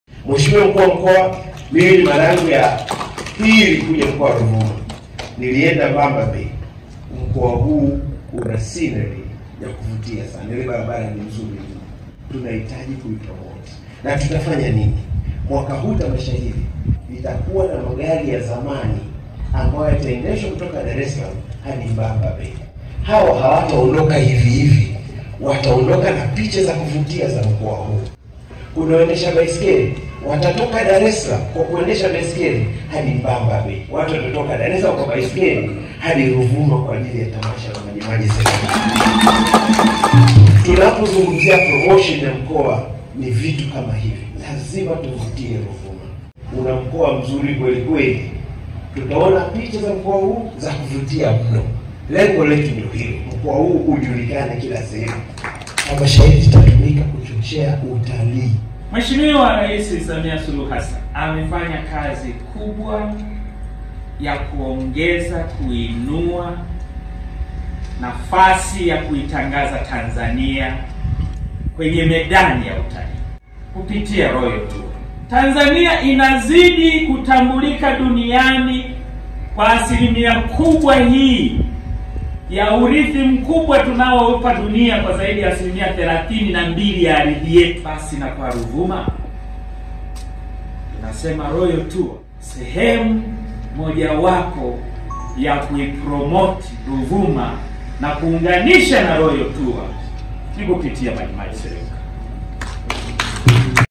THE ROYAL TOUR INAVYOUNGANISHWA NA MAJIMAJI SELEBUKA
Mkuu wa Mkoa wa Ruvuma Brigedia Jenerali Wilbert Ibuge amefungua tamasha la Majimaji Selebuka kwenye viwanja vya Makumbusho ya Taifa ya Majimaji mjini Songea.Tamasha hilo lilianzishwa mwaka 2014 na mwaka huu ni tamasha la nane kufanyika mkoani Ruvuma ambalo linatarajia kuanza Julai 23 hadi 30 mwaka huu.